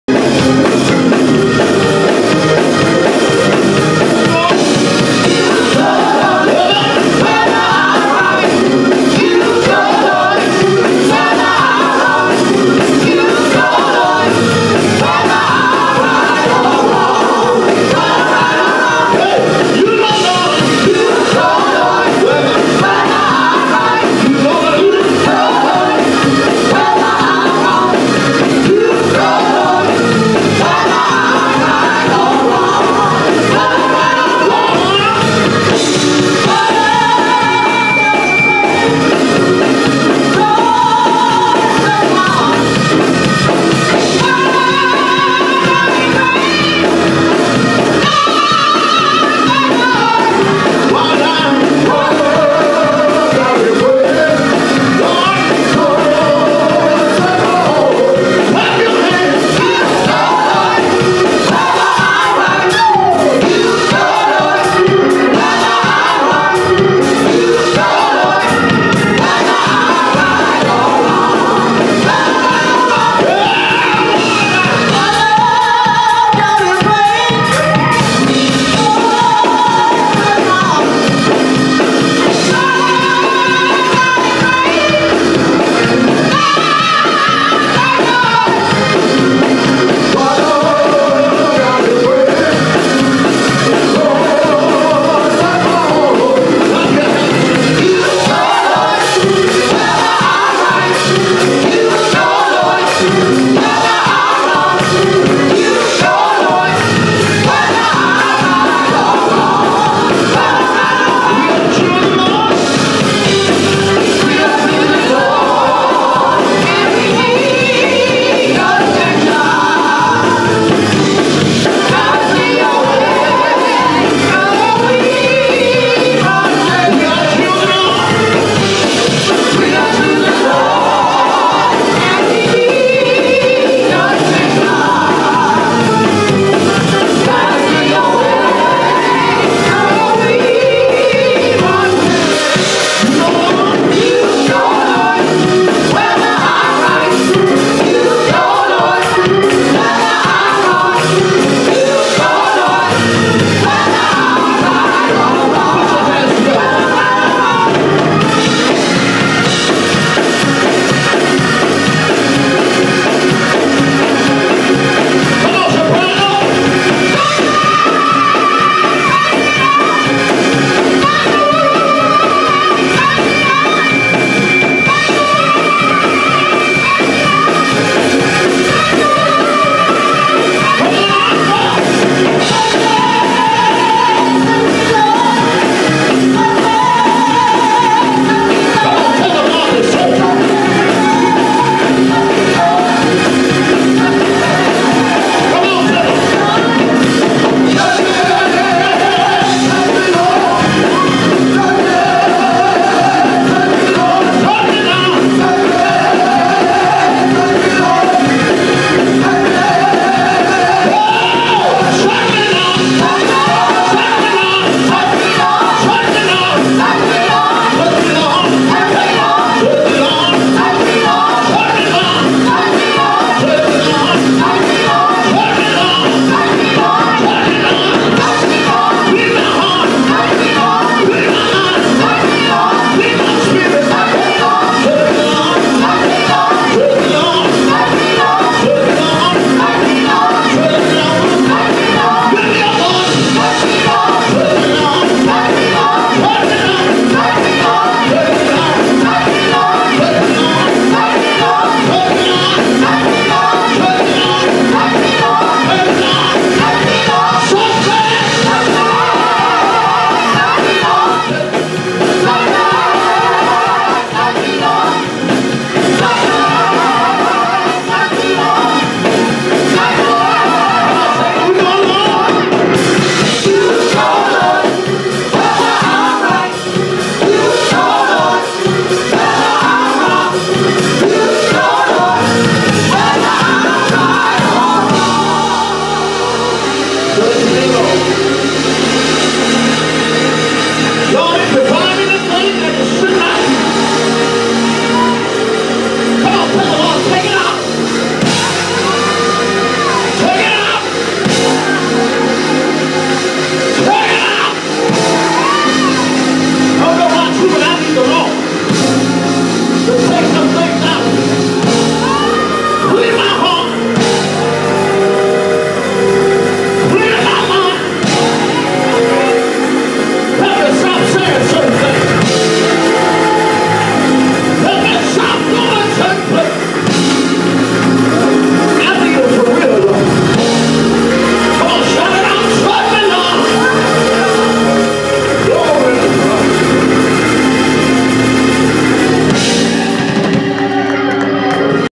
The Worship Experience 10.25.2020
Talented musicians and singers led by music Minister Shaquille Davis